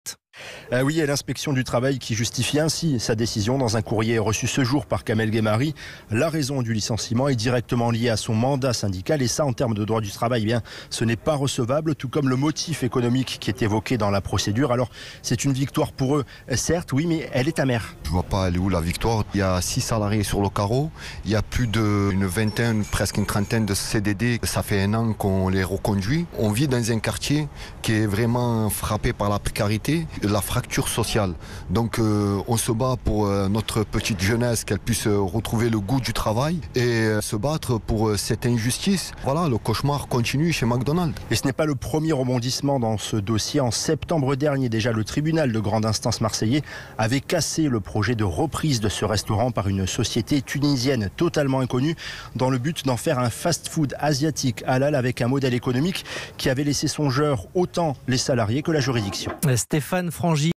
MARSEILLE : LE LICENCIEMENT DU LEADER DU COMBAT DES "MCDOS" REFUSÉ PAR L'INSPECTION DU TRAVAIL
Marseille : l'inspection du travail refuse le licenciement du leader du combat des "McDos"